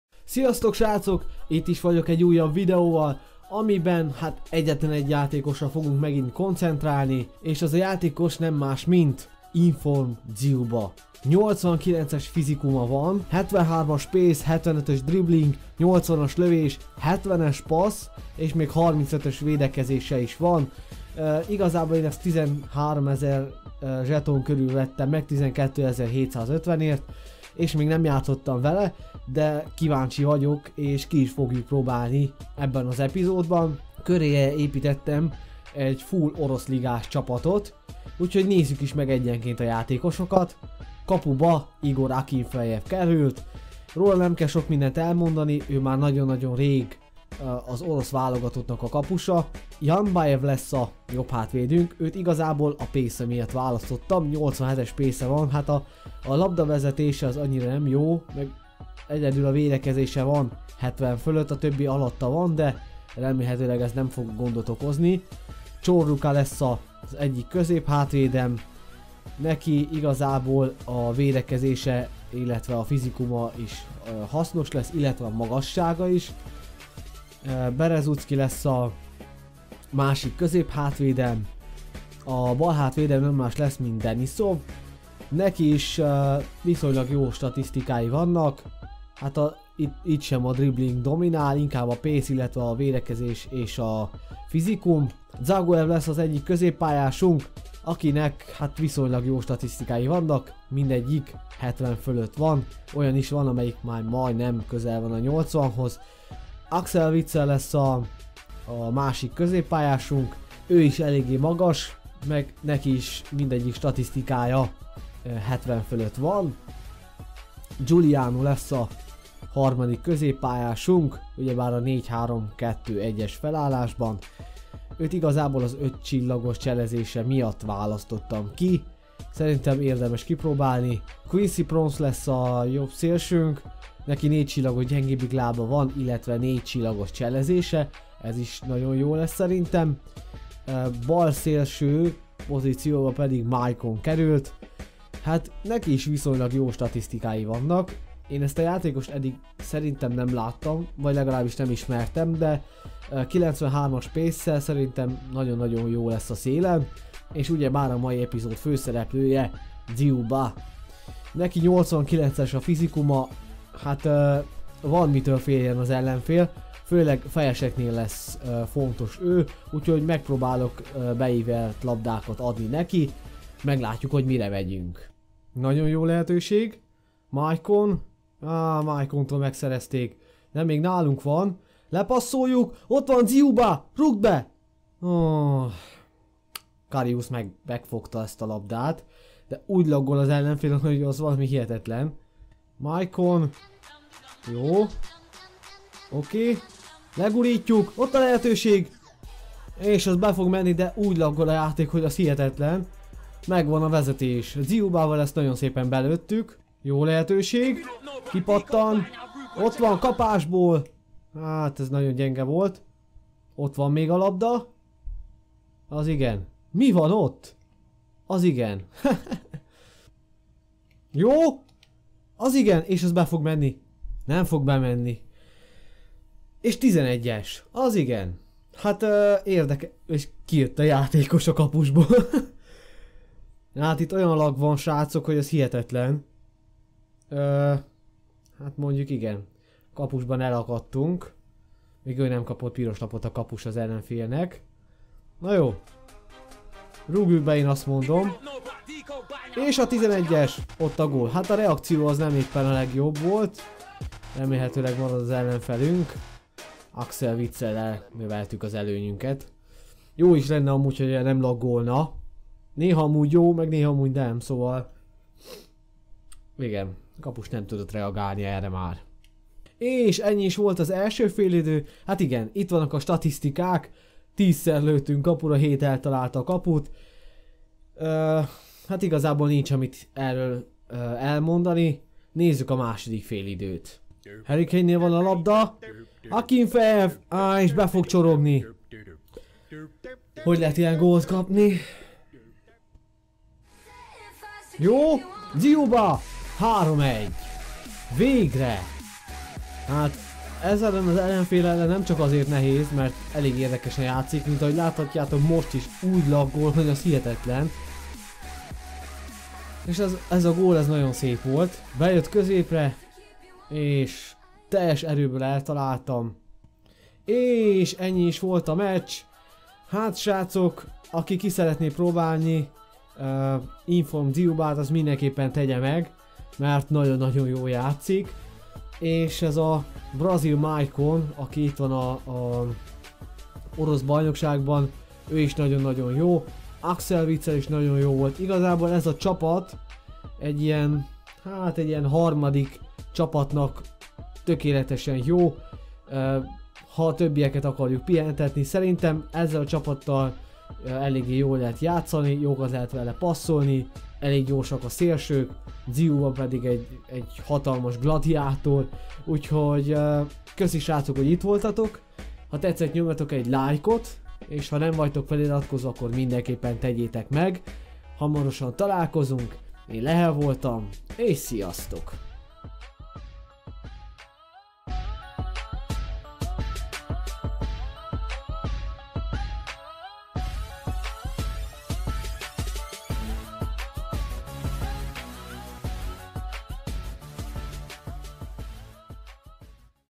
AZ OROSZ GLADIÁTOR - IF DZYUBA SQUAD BUILDER!!
Kipróbáltam Inform Dzyuba kártyáját, hát ez eszméletlen jó!!! Ha kíváncsi vagy milyen volt a meccs nézd meg a videót! :)
Jó szórakozást! :D